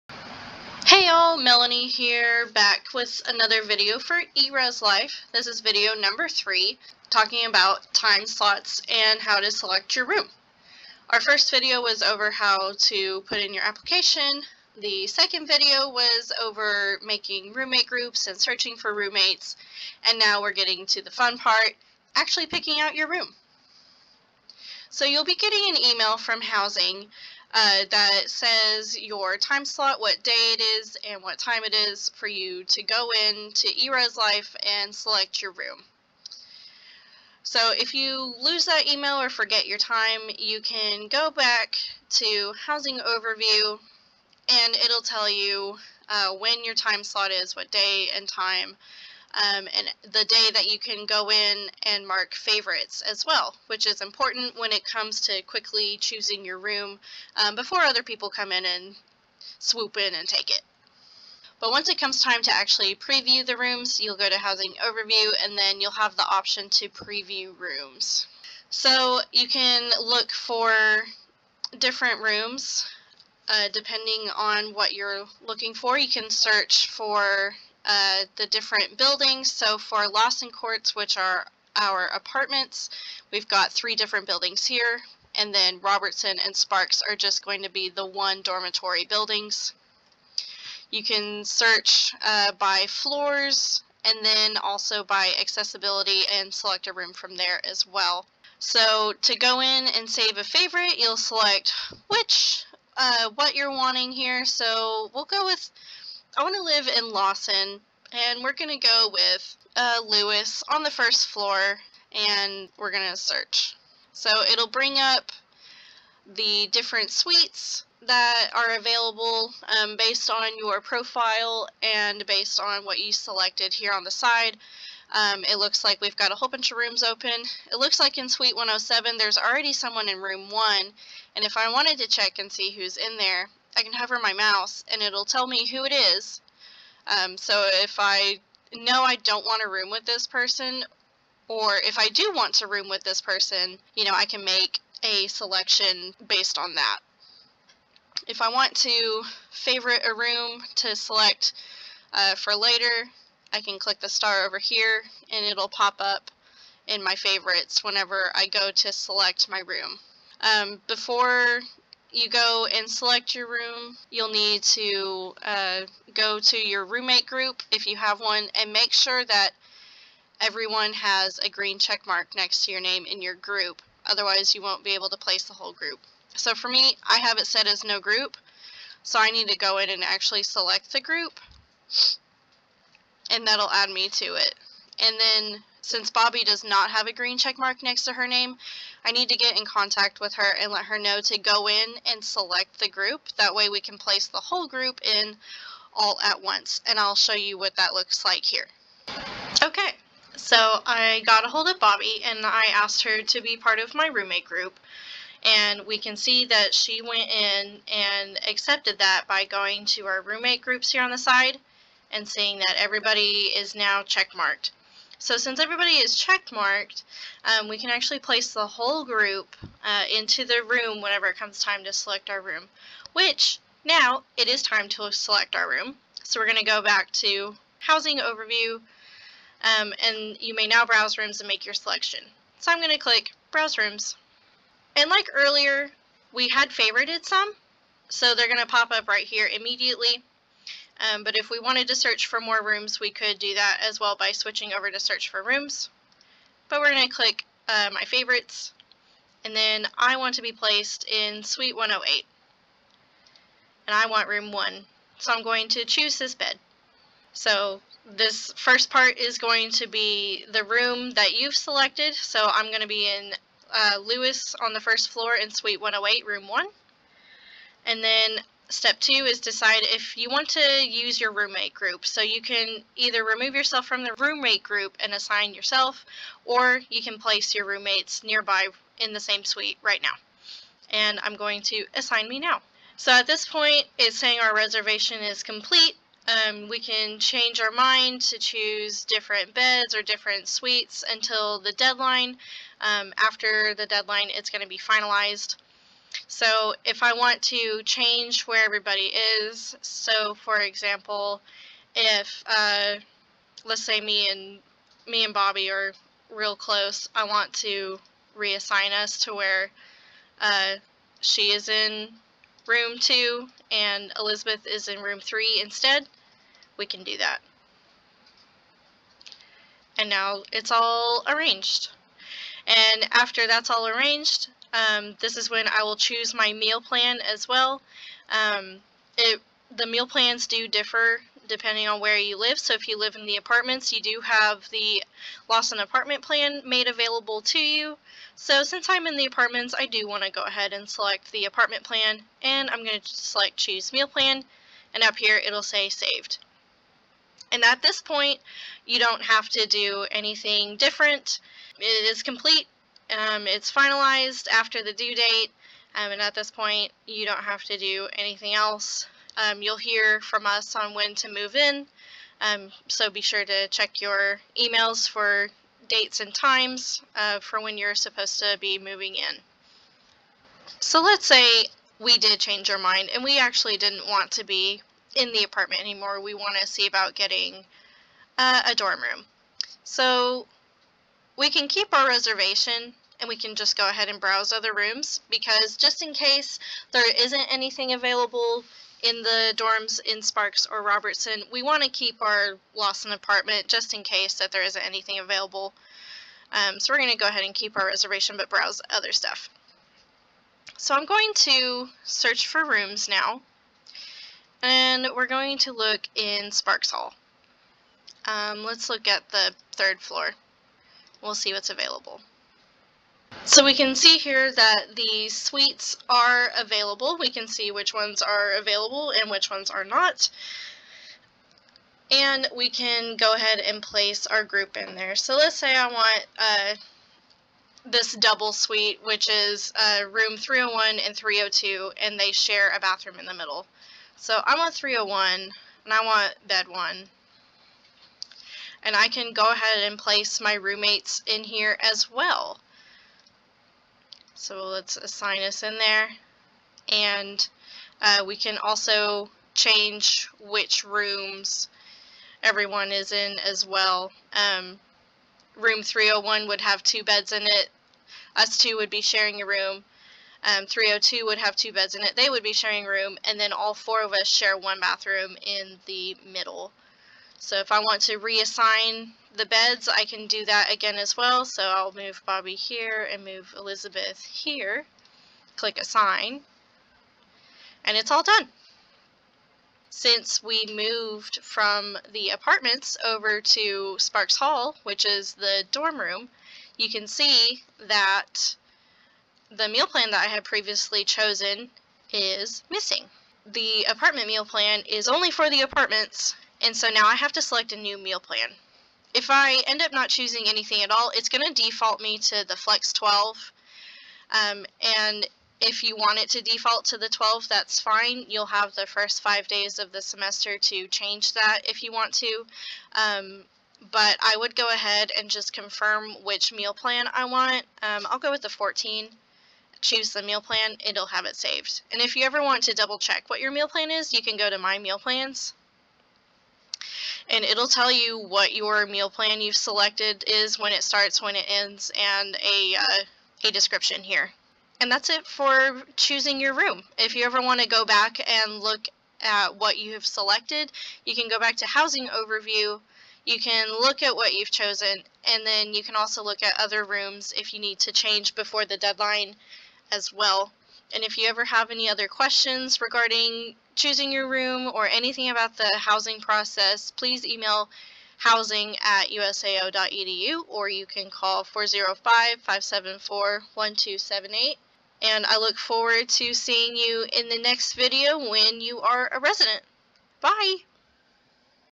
Science & Arts | Room selections
Student Services walks you through making room selections at the University of Science and Arts of Oklahoma.